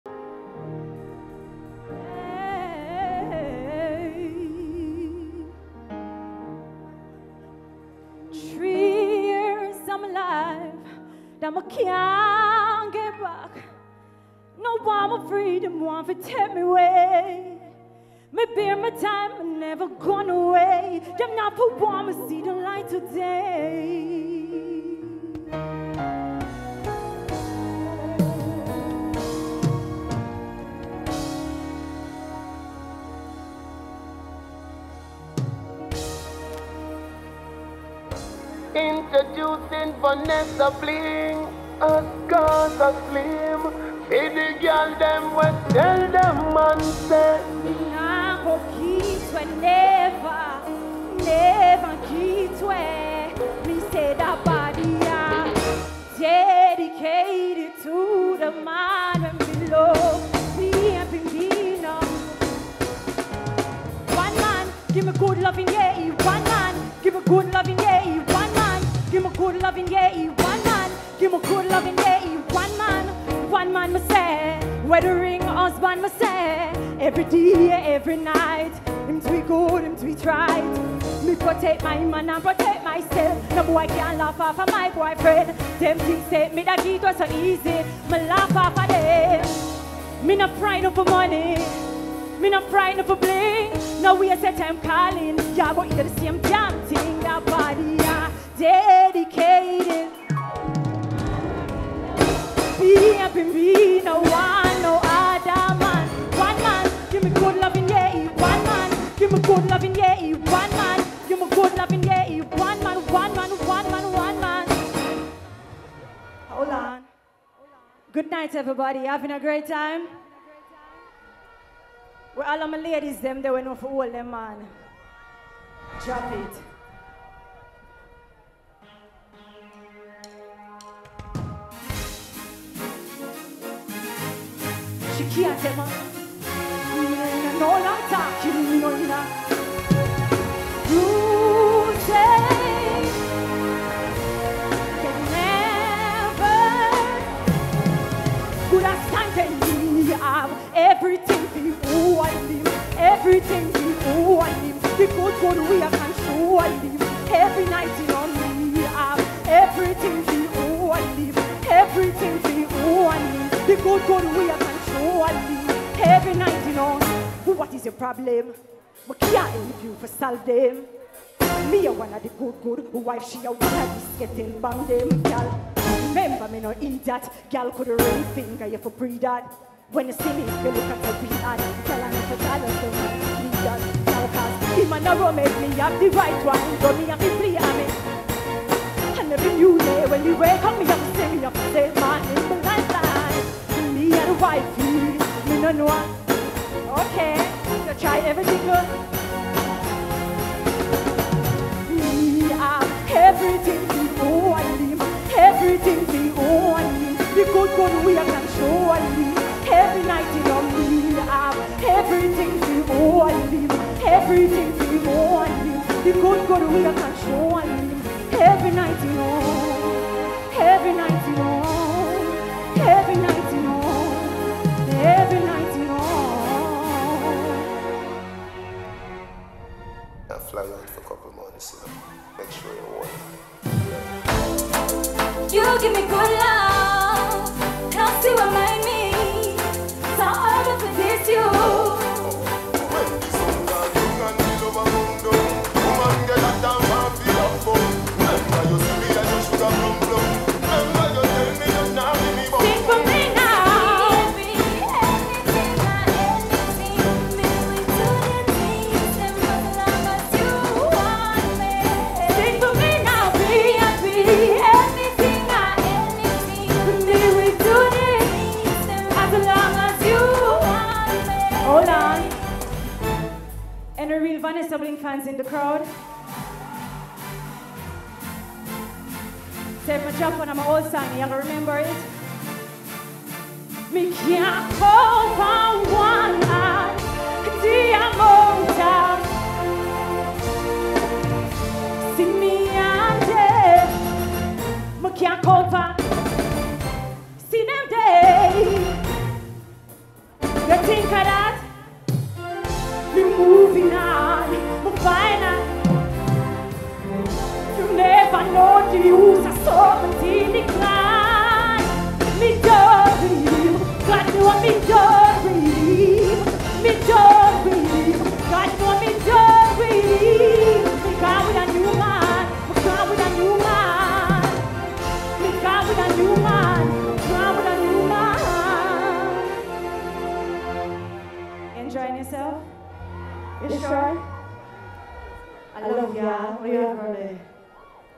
Vanessa Bling - Reggae Sumfest 2023 (Part 1 of 3)
Reggae Sumfest 2023 | Dancehall Night
Friday, July 21st

Executive Producer - Joseph Bogdanovich
Producer - Sepp Donahower
Production Services  - Phase 3 Productions